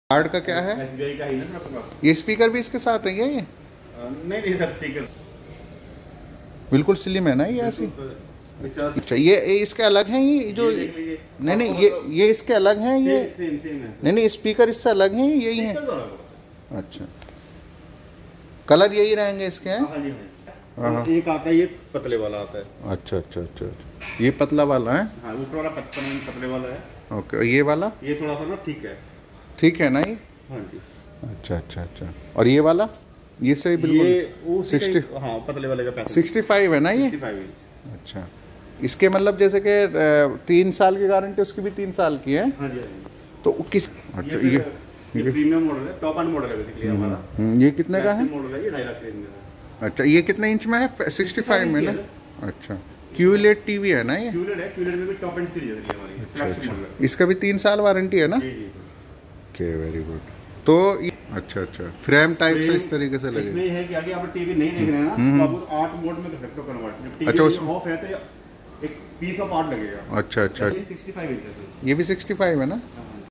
Samsung Led Tv 43 Inch Review 4k - Samsung Gled Tv 55 Inch Price In India Samsung Vlog
About This Video - Doston, aaj ke video mein hum karne wale hai quick review Samsung Crystal 4K Pro TV ka. Ye TV aata hai kuch kamaal ke features ke sath, to aap ye video ant tak jaroor dekhiye.

This video has been made in association with Samsung. However, all views expressed in this video are our own!

entries which don’t comply will be disqualified.